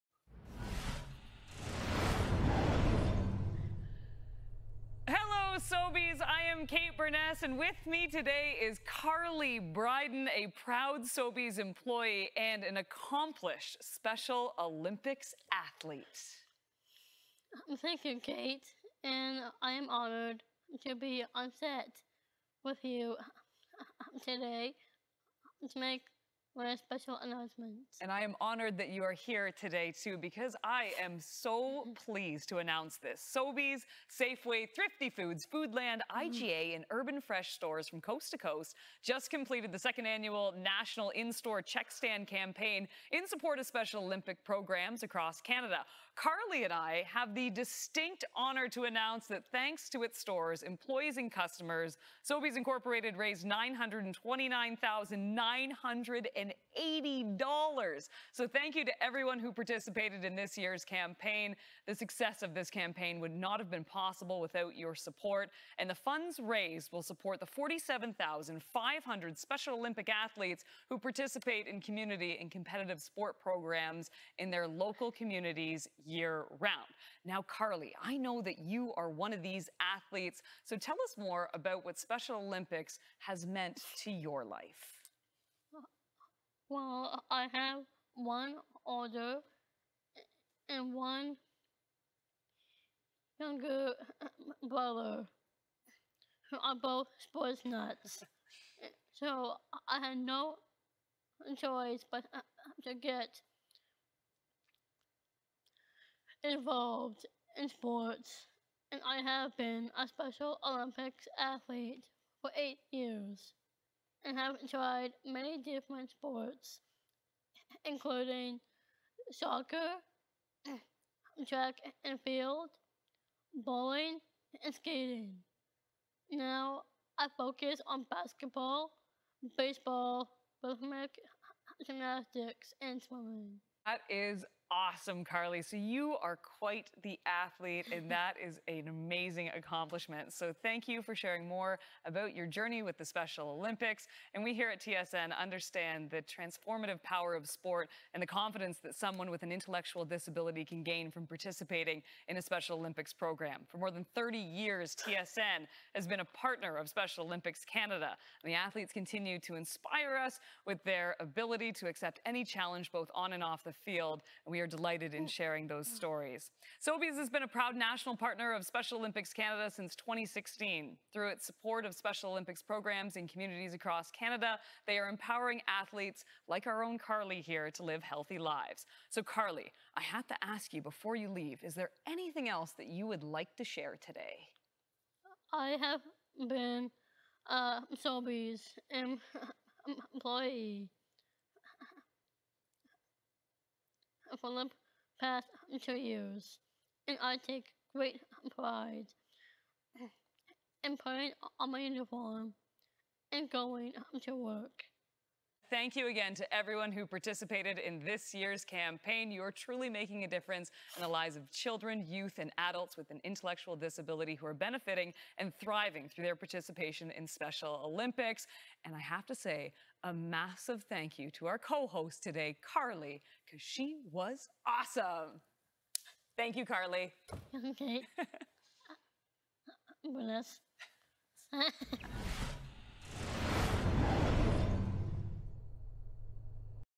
Sobeys customers raised $930,000 for Special Olympics athletes
Did you donate $2 at Sobeys, Canada Safeway, Thrifty Foods, IGA, Urban Fresh or Foodland last month?

You helped raise an impressive $929,980 for Special Olympics athletes across the country!

Who better to say thank you than Special Olympics Ontario athlete/Sobeys employee Carly Bryden alongside TSN’s Kate Beirness?
Avez-vous fait un don de 2 $ à la caisse d’une épicerie Sobeys, Safeway, Thrifty Foods, IGA, Urban Fresh ou Foodland le mois dernier?

Vous avez contribué à amasser l’impressionnante somme de 929 980 $ au profit d’athlètes d’Olympiques spéciaux partout au pays!

Qui de mieux pour vous remercier que Carly Bryden, athlète d’Olympiques spéciaux Ontario et employée de Sobeys, aux côtés de l’animatrice de TSN Kate Beirness?